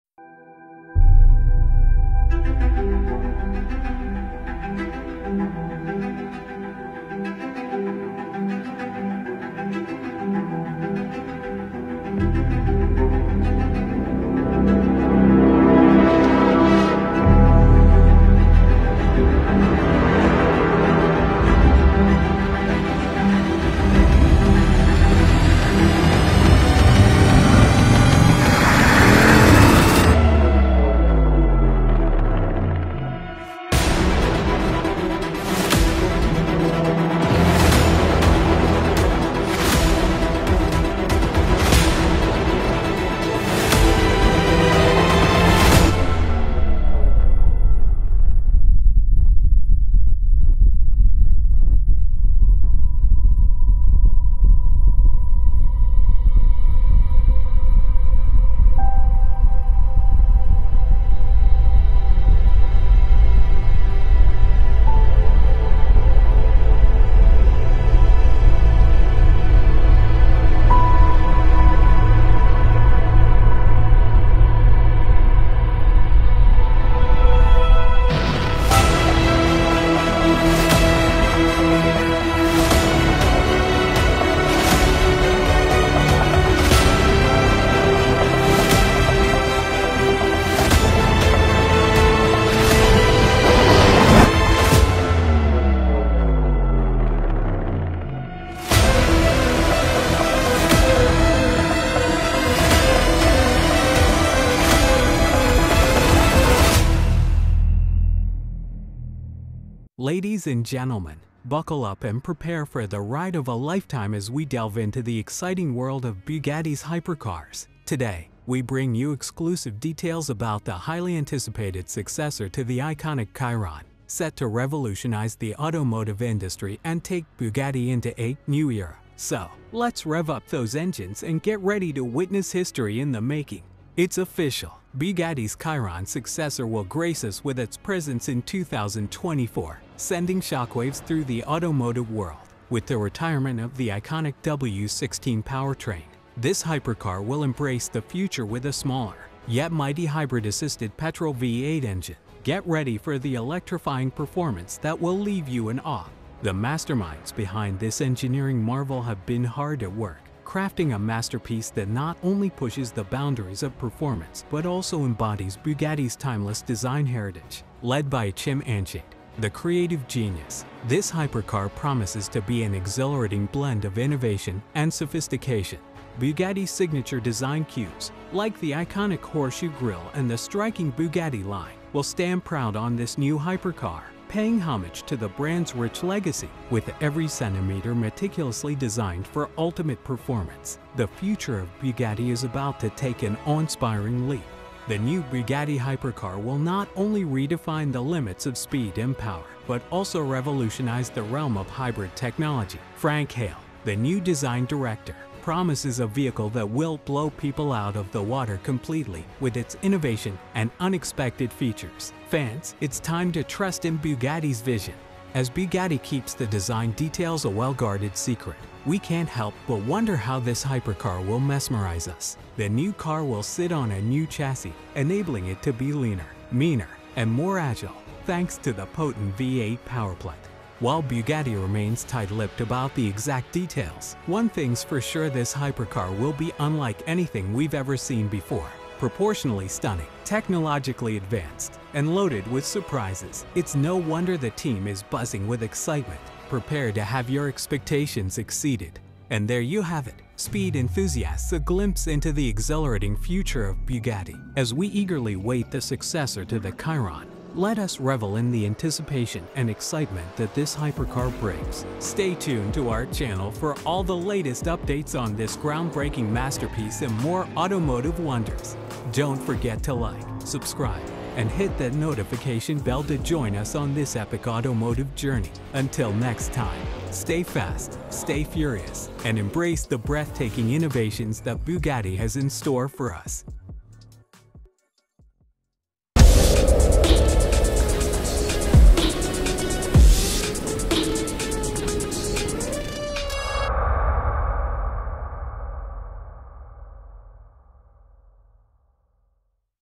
NEW Bugatti Chiron's Successor Hypercar Revolution 2024
As Bugatti prepares to unveil the replacement for the venerable Chiron, get ready for a thrilling voyage into the future of hypercars. Exclusive leaks and intimate knowledge are presented in this engrossing YouTube movie, giving you a glimpse at the features and design of Bugatti's next hypercar, due to be unveiled in 2024.

Random Tags:
alfaromeo giulia